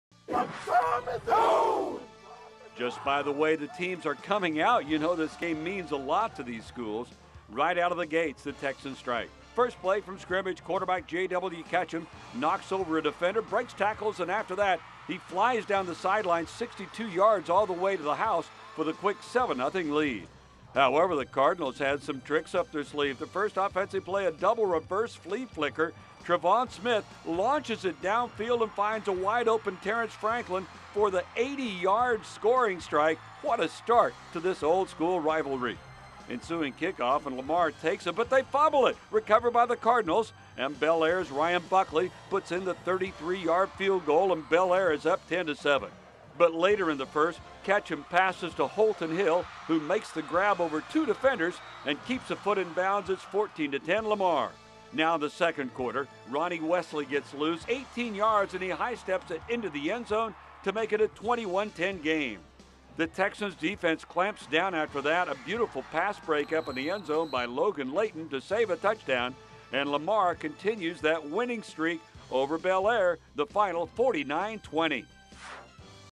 Week 7-Lamar Texans vs. Bellaire Cardinals-2014 Football
Just by the way the teams were coming out…you know this game means a whole lot to these schools
and right out of the gates…the Texans strike….1st play from scrimmage, quarterback JW Ketchum knocks over a defender breaks tackles and after that…he just flew down the sidelines…62 yards all the way to the house for the quick 7-0 lead….
However…The Cardinals had some trick up their sleeve…their first offensive play…a double reverse flea flicker…Trevon Smith launches it down the field and he finds a wide open Terrence franklin for the 80 yard score…what a start in this old school rivalry…
Ensuing kickoff…Lamar takes it but they fumble it, recovered by the Cardinals…Bellaire’s Ryan Buckley puts in the 33 yd fg and it was 10-7 Bellaire…
But later…end of the 1st quarter…Ketchum gets it to Holton Hill who makes the grab over two defenders and keeps a foot in bounds…14-10 Lamar…
2nd quarter…former athlete of the week Ronnie Wesley…right up the gut…18 yards and he high steps it into the endzone to make it 21-10…
The Texans defense clamped down after that…beautiful pass break-up in the endzone by Logan Latin to save a touchdown…
And Lamar continues that winning streak over their hated rivals…